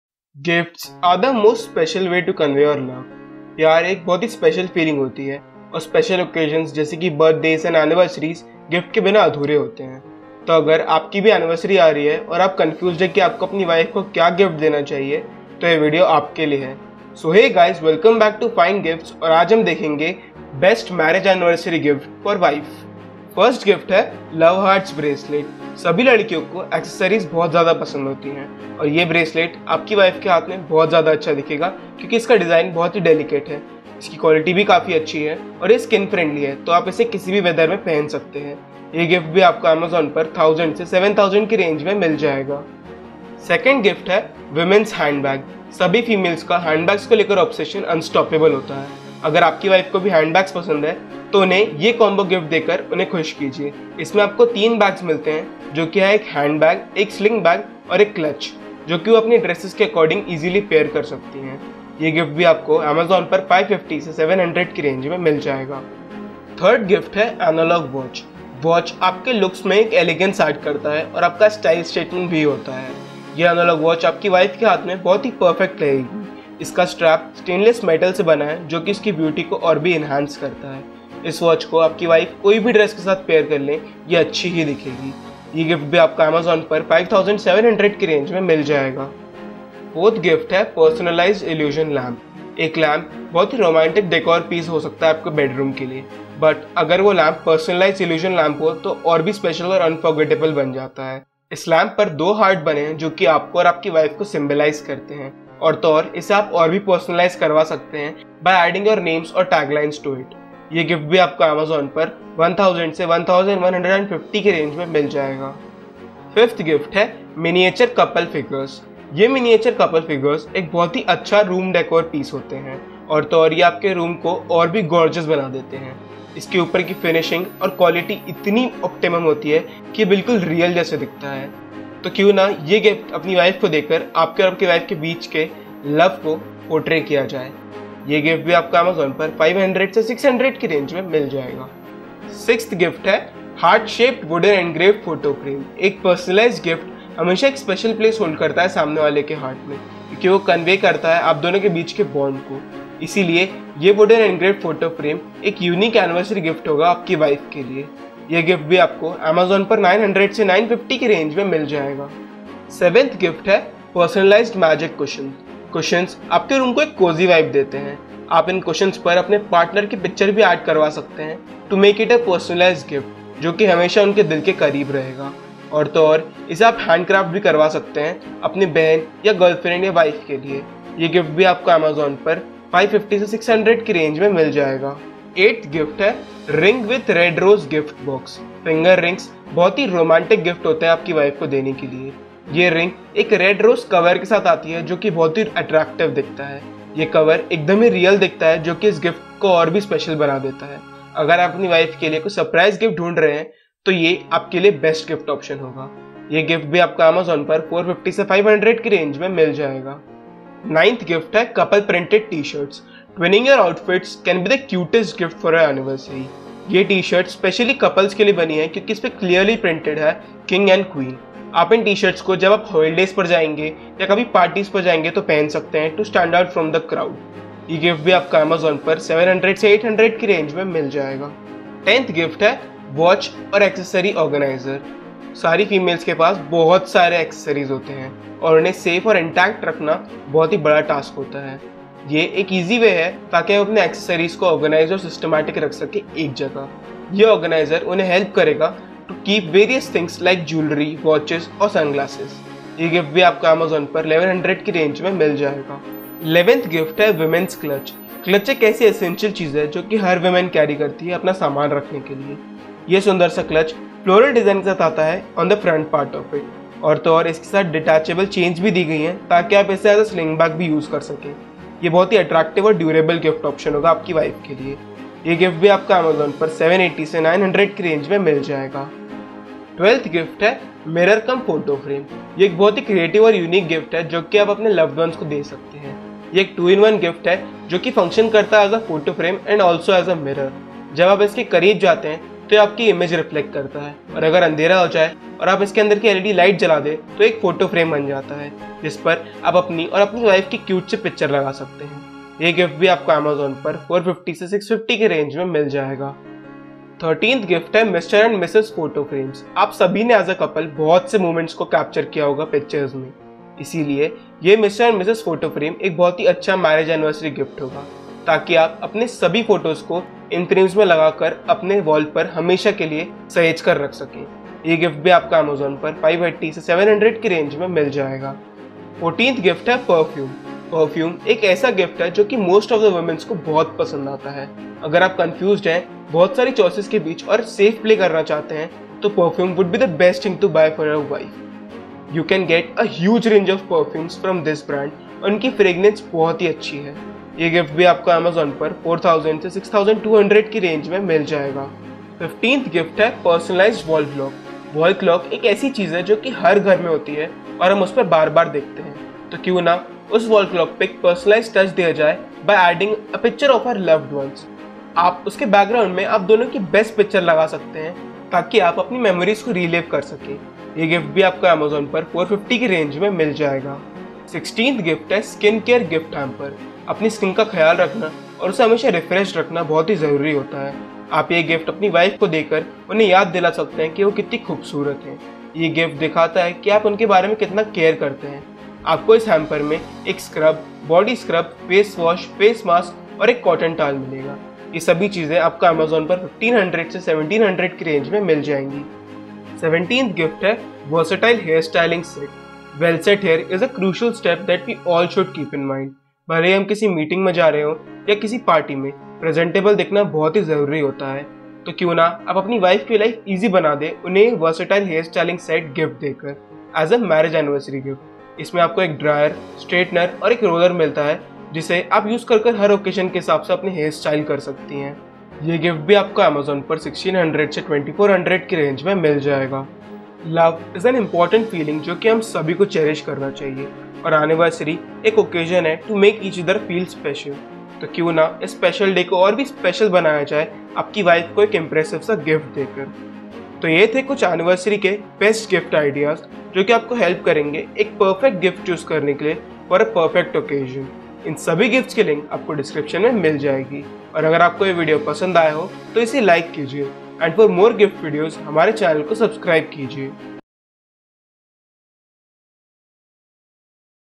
Best 17 Anniversary Gift for Wife in India (2023) || Best Anniversary Gifts for Wife
Gifts are the most special way to convey your love. Love is the most special feeling in the world and special occasions like birthdays and anniversaries call for a memorable gift. Coming up with a unique and thoughtful gift for your wife is not a simple task. Your wife holds a special place in your heart and has made your life beautiful so her gift has to be something that compliments her importance in your life.

The best marriage anniversary gift is something that would make her jump in excitement. But as you all know pleasing girls is not so easy, her choice and taste may differ from time to time so you need to be updated about her likes and dislikes. Here are a few shortlisted gifts that are incredible and worth buying.

Best 17 Anniversary Gift for Wife in India For 2023 which you can buy online from Amazon & Flipkart

1) Love Hearts Bracelet

2) Women’s Handbag

3) Analog Watch

4) Personalized Illusion Lamp

5) Miniature Couple Figurines

6) Heart-Shaped Wooden Engraved Photo Frame

7) Personalized Magic Cushion

8) Ring With Red Rose Gift Box

9) Couple Printed T-Shirts

10) Watch/Accessory Organizer

11) Women’s Clutch

12) Mirror Cum Photo Frame

13) Mr & Mrs. Photo Frames

14) Perfume

15) Personalized Wall Clock

16) Skin Care Gift Hamper

17) Versatile Hair Styling Set

⌛TIMESTAMPS
00:00 Intro
00:24 1) Love Hearts Bracelet
00:42 2) Women’s Handbag
1:06 3) Analog Watch
1:28 4) Personalized Illusion Lamp
1:57 5) Miniature Couple Figurines
2:22 6) Heart-Shaped Wooden Engraved Photo Frame
2:43 7) Personalized Magic Cushion
3:05 8) Ring With Red Rose Gift Box
3:33 9) Couple Printed T-Shirts
3:53 10) Watch/Accessory Organizer
4:18 11) Women’s Clutch
4:37 12) Mirror Cum Photo Frame
5:07 13) Mr & Mrs. Photo Frames
5:29 14) Perfume
5:52 15) Personalized Wall Clock
6:15 16) Skin Care Gift Hamper
6:39 17) Versatile Hair Styling Set